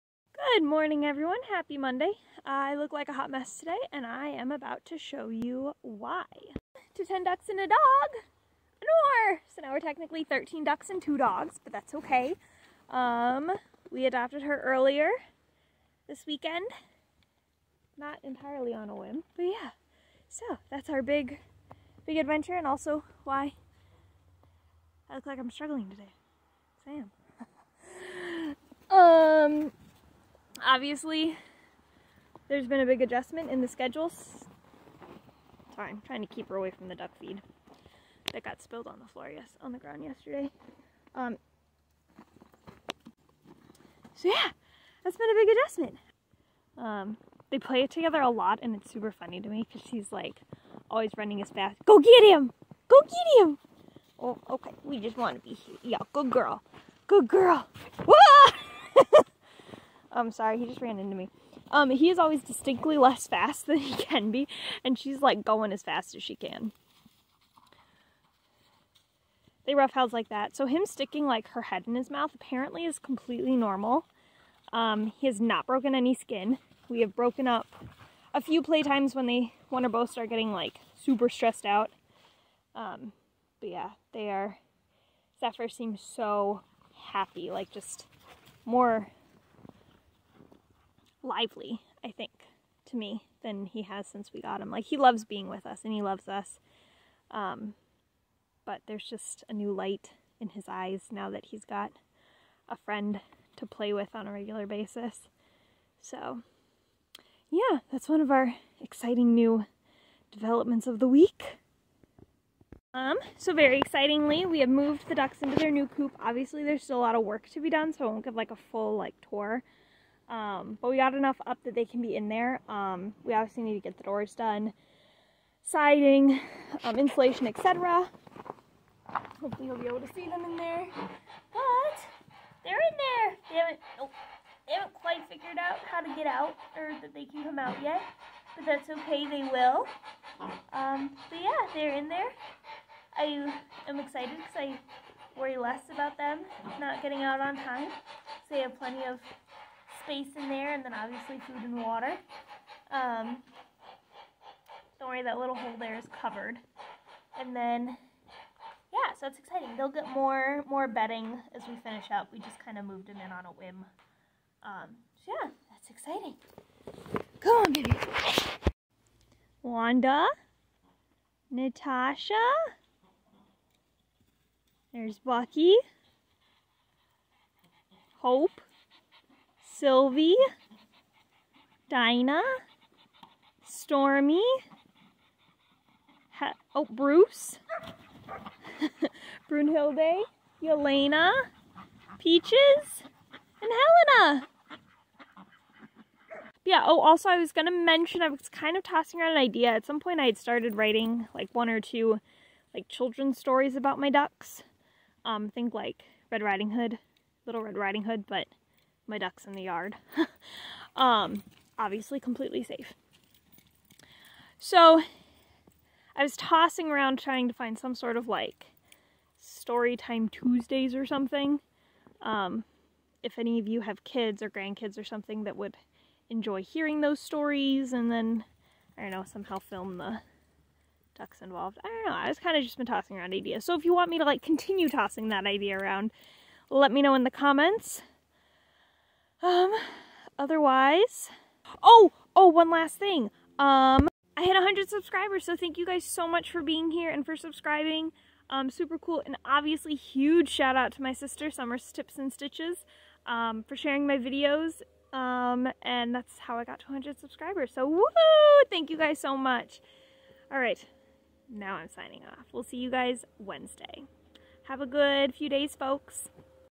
Quack Chats 3
Welcome, Anore!

Zephyr is SO happy to have a friend, and we're so happy to have added our second (of hopefully four) winds!

Zephyr means West wind, Anore means wind, and the next few dogs we get will also be wind themed as well! Anore is actually a boys name in Greenlandic, but I thought it was a beautiful girls name!

HUGE thanks to Summer's Tips and Stitches and YOU for being here with me, as I've reached 100 subscribers! Woo!! thank you folks so much!!